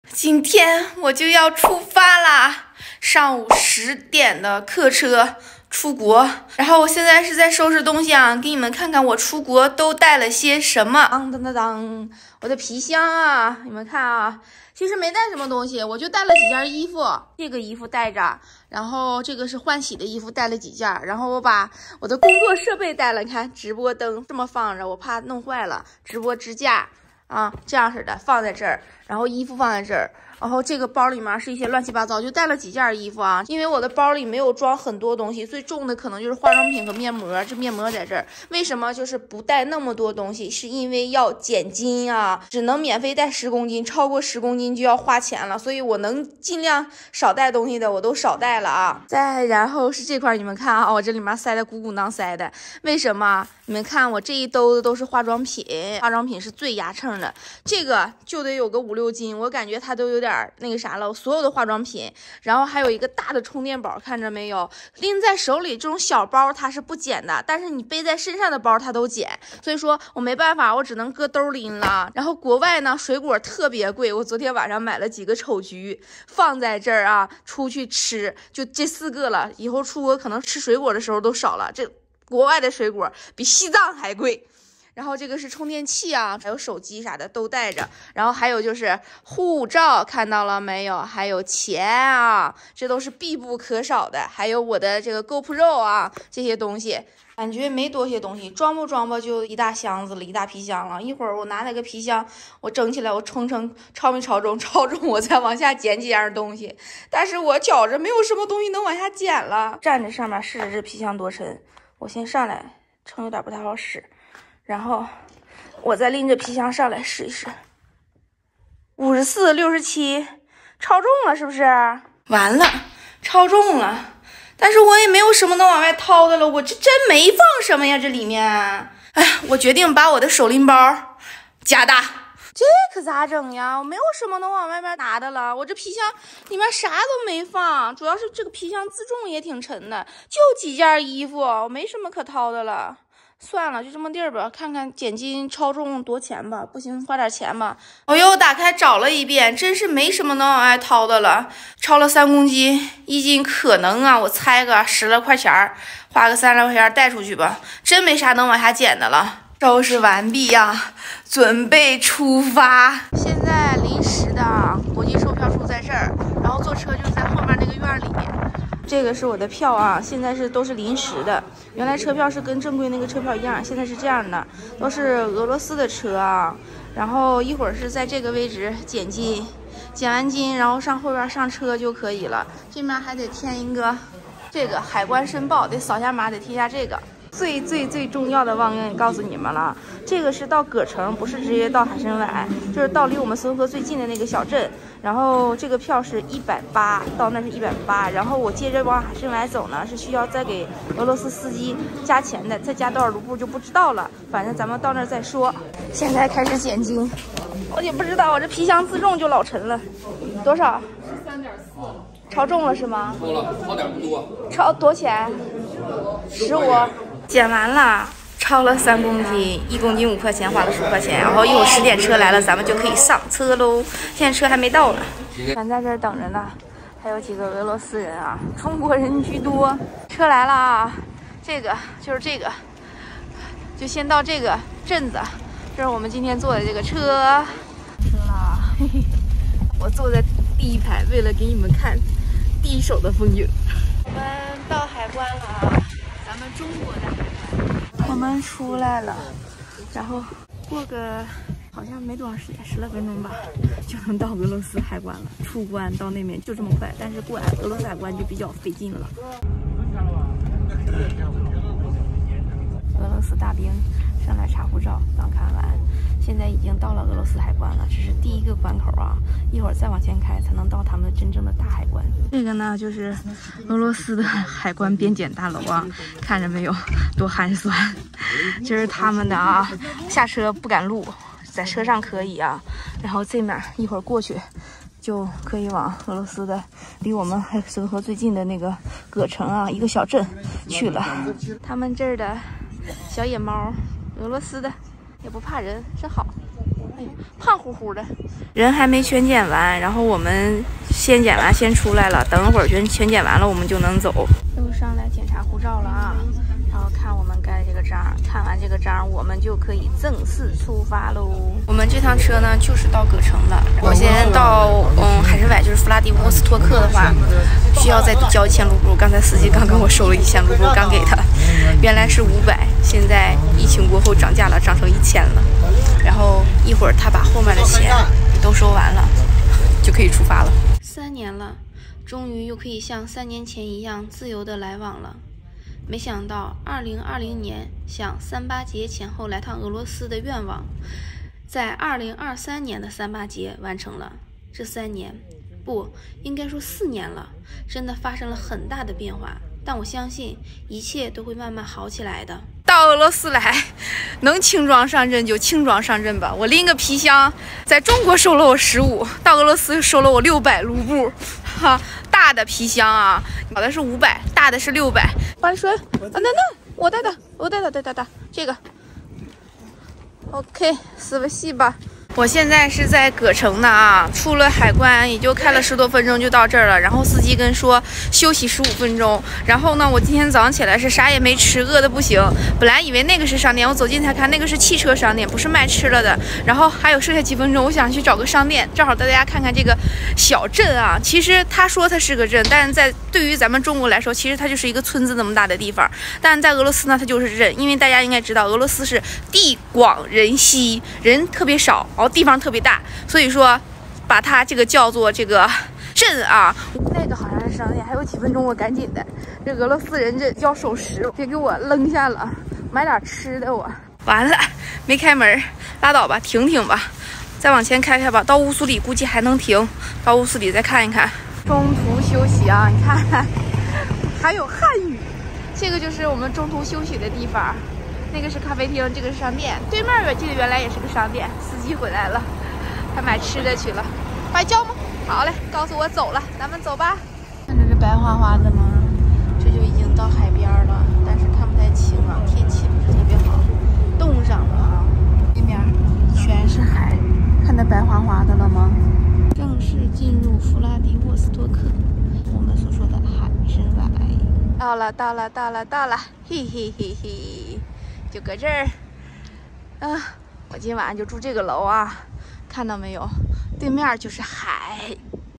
坐国际大巴车入境俄罗斯，时隔三年又踏上这片土地，心情激动！【东北小鱼】
坐国际大巴车入境俄罗斯，时隔三年又踏上这片土地，心情激动！
东北小鱼 俄罗斯 旅行生活 中文频道 中国大陆 我们强调了视频内容的专业性和安全性，以确保观众获得可靠的信息。